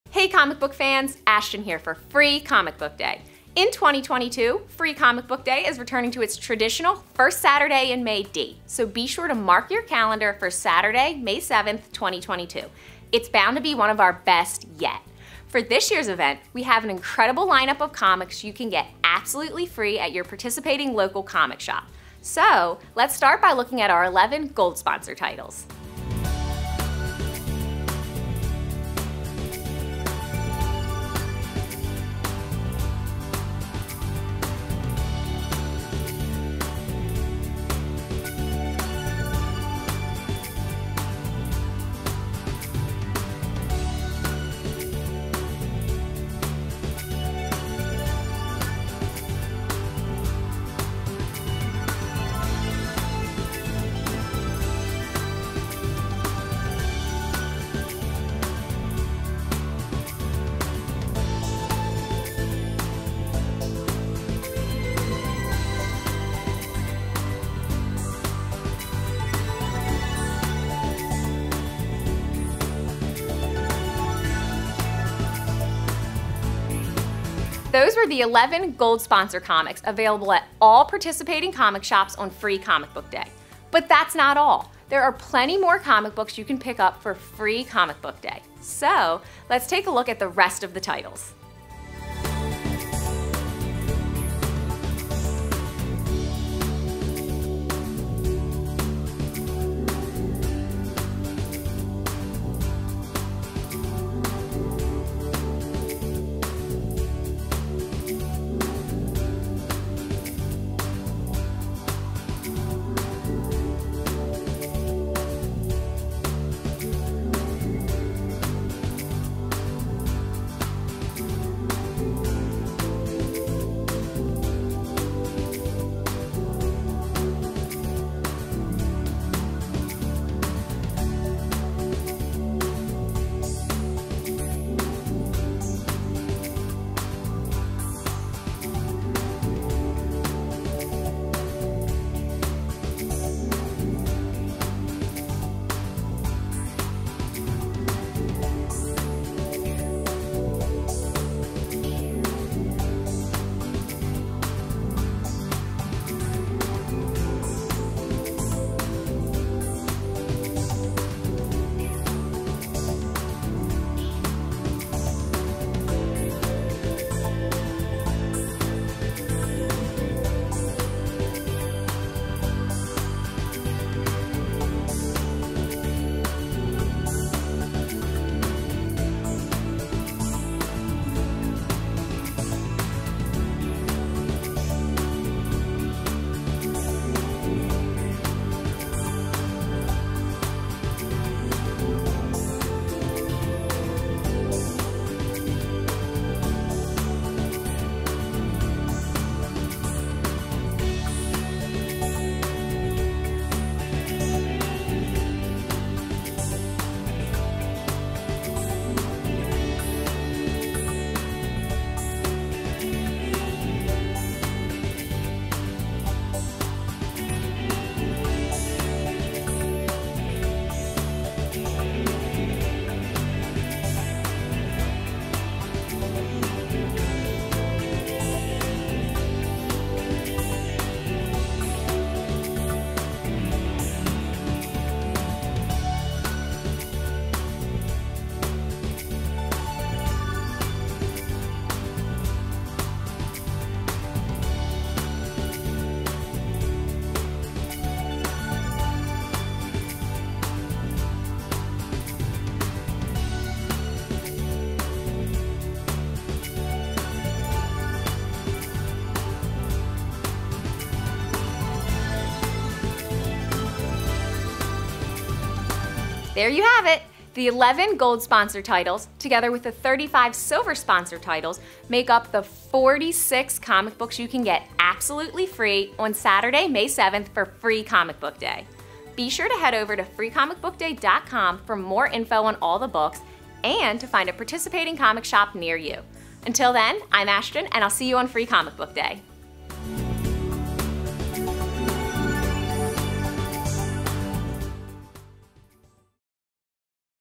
Full Line-Up of FCBD 2022 Comic Books Announced!
Watch now as we reveal the full list of comics for Free Comic Book Day 2022 with a lineup that features 46 outstanding titles for fans! Get more details on all the books